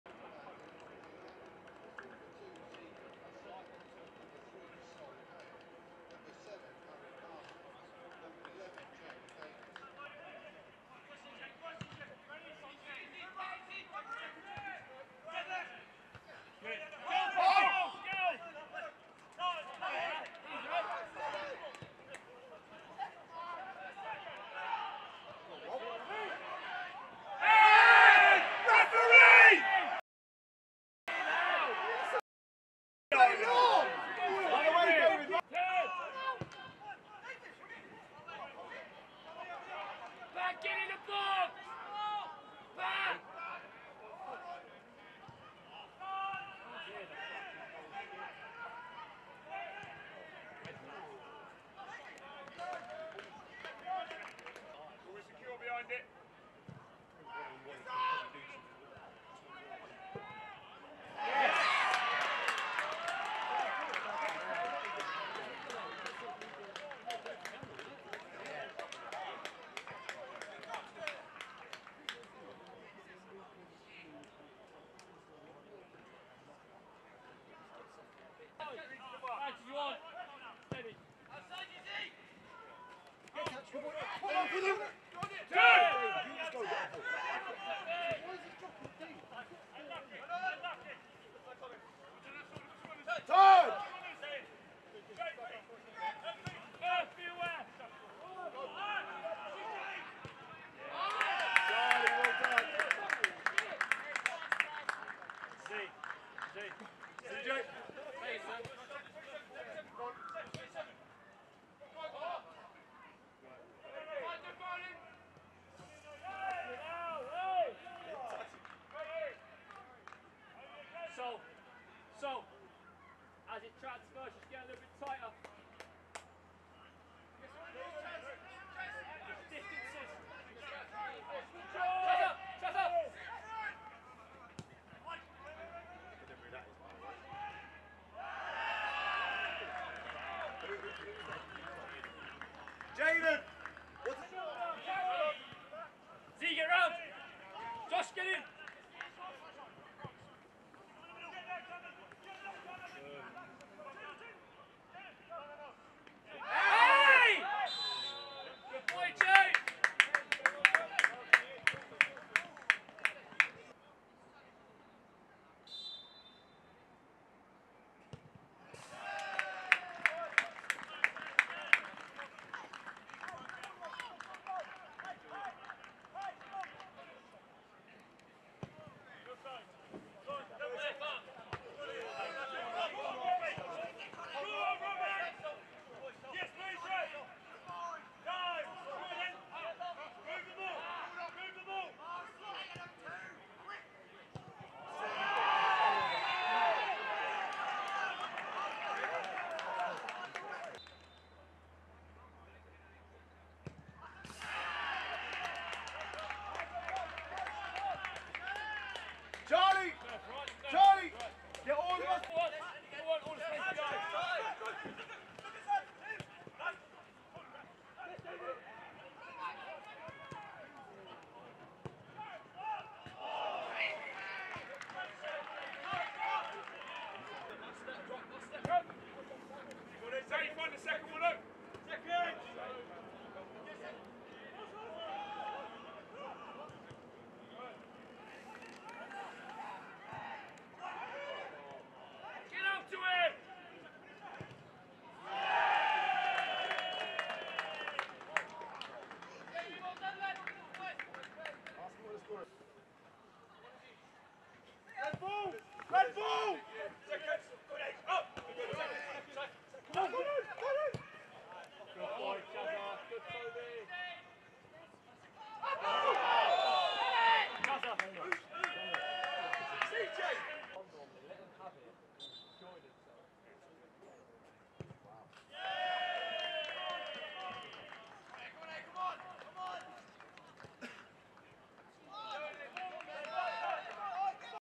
Highlights: Didcot Town vs Maidenhead United
The key events from Loop Meadow as Diddy hosted National League Maidenhead United in the Berks and Bucks County Cup